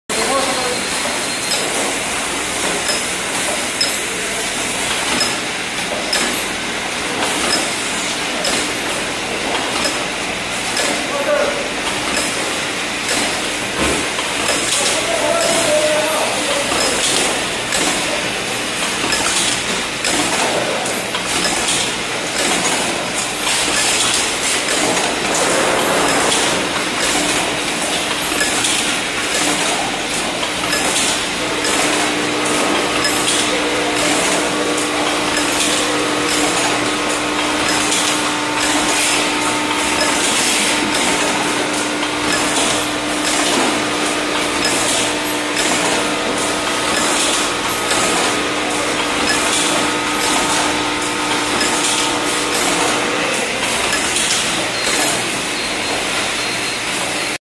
Bisignano S.A. DLB R2|B Liquid Filling Line
BISIGNANO S.A. DLB R2|B LIQUID FILLING LINE | Listing #529526

- Model: DLB R2/B
- Year: 2011
- Almost new
- Automatic machine with Allen Bradly HPLC
- Automatic feeder with caps for Portola 38 mm snap on caps.
- Used with drinking yogurt bottles, but it can be used all types of liquids
- Bought new to the manufacturer
- Includes manual and pieces for 100cc and 190cc bottles